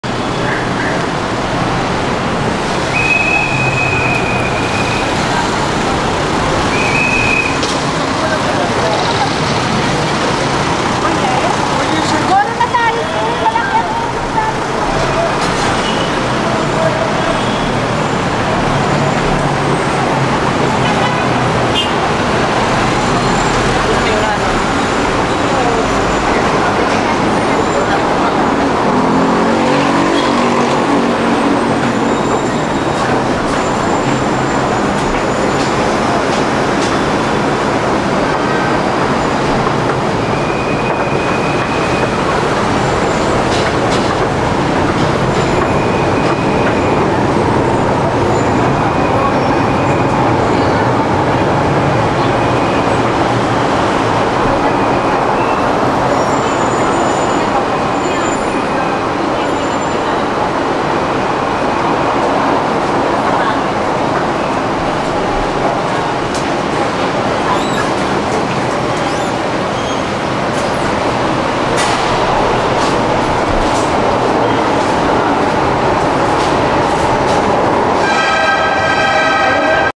Philippines, Quezon City, escalator ride @ Quezon Avenue MRT station
escalator 0:28

- recorded with Sony a6000, Sony SEL1018, Zhiyun Crane v1.0

Auto-Fix applied

October 2017